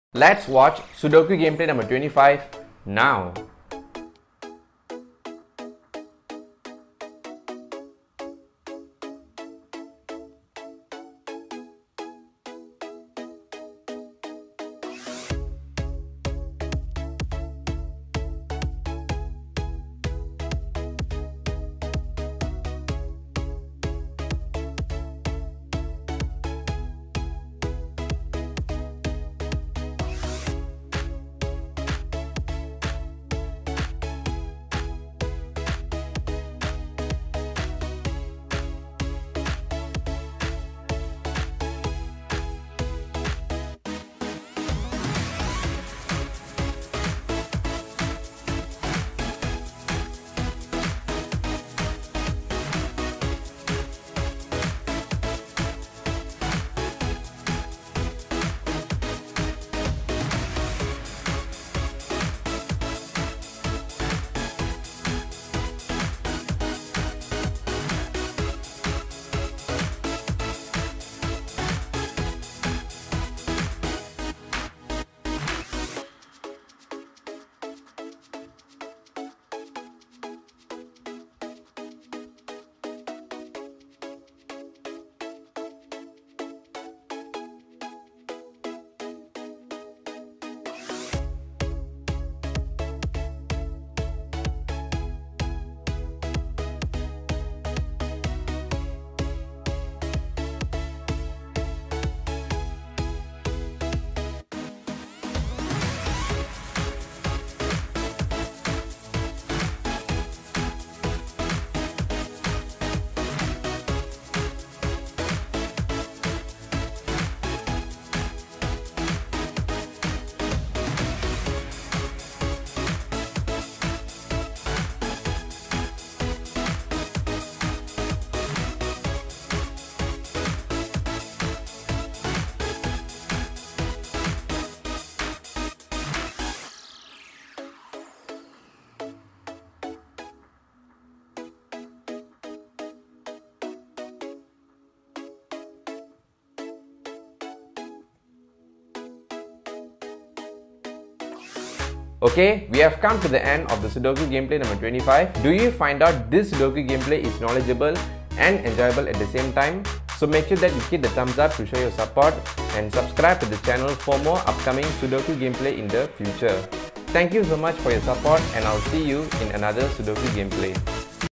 Let's Play Sudoku Easy Level in 2021 | Math Tutor Edu | Sudoku #25
This is the 25th video sudoku live let's play easy level in the year 2021 (sudoku #25). This is an initiative from math tutor edu where sudoku is actually easy to be solved by general people regardless of age (sudoku for everyone in shortly) . Hope you guys will enjoy these gameplay of sudoku easy level for beginner in 2021.

This is the latest sudoku video in the year 2021.